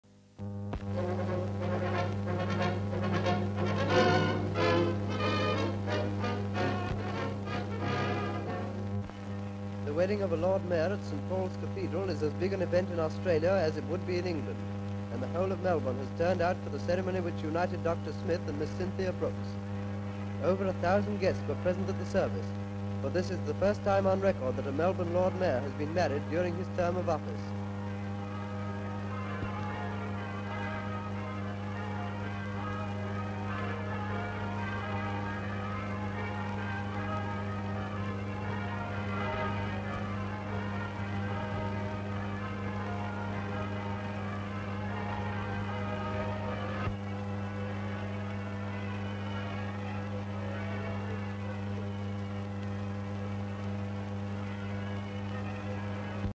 AUSTRALIA: Lord Mayor of Melbourne wedding (1934)
Wedding of Doctor Harold Gengoult Smith, Lord mayor of Melbourne

Full Description:

SLATE INFORMATION: Melbourne Ceremony: Huge Crowds Attend Wedding of Lord Mayor

AUSTRALIA: Melbourne:
Wedding of Lord Mayor of Melbourne, Doctor Harold Gengoult Smith, and Miss Cynthia Brookes at St Paul's Cathedral including huge crowds

Australia; Rites of Passage; Personalities - Local Government
Background: Wedding of Doctor Harold Gengoult Smith, Lord mayor of Melbourne